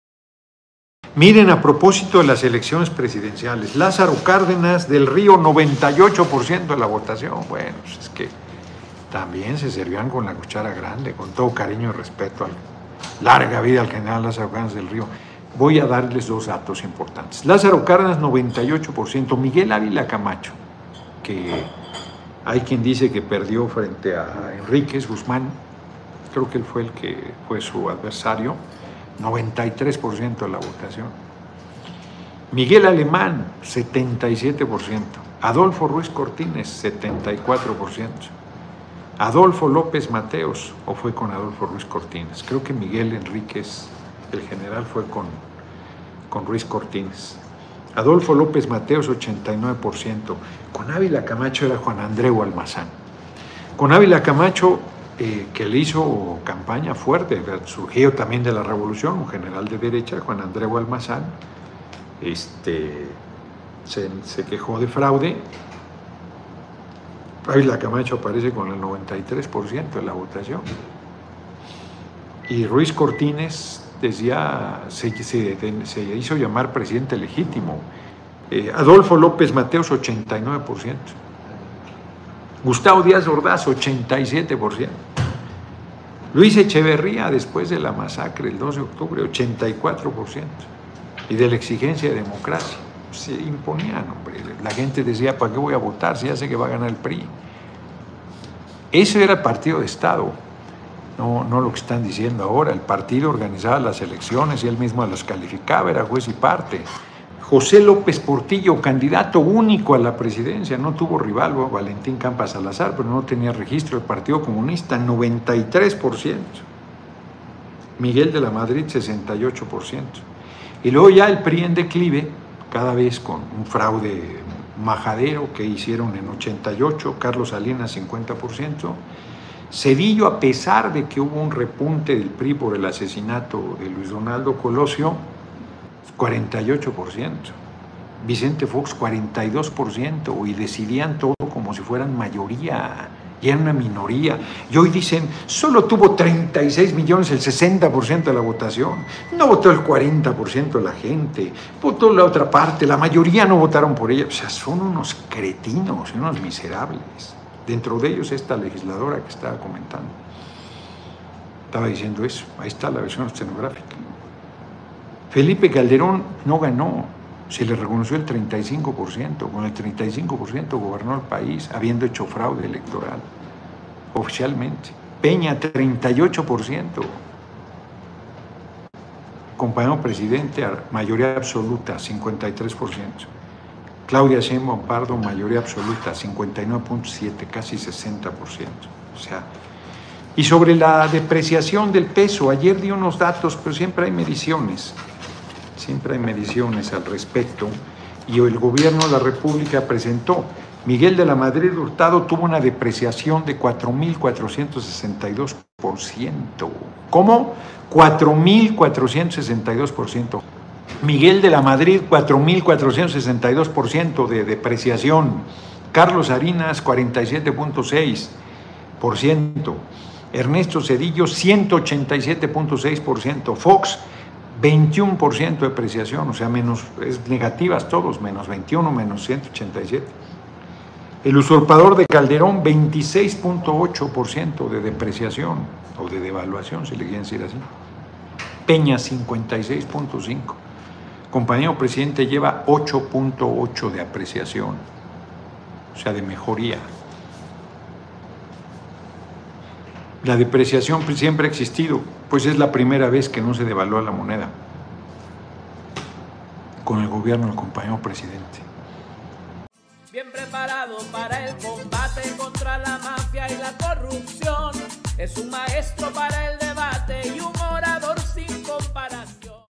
Les voy a dar dos datos importantes
Muchas gracias por participar con nosotros compartiendo los mensajes del Lic.

Fragmento de la videocharla del Diputado y coordinador de enlace con organizaciones sociales y civiles de la Dra. Claudia Sheinbaum Pardo, Gerardo Fernández Noroña correspondiente al día 12 de junio de 2024, puedes ver nuestro resumen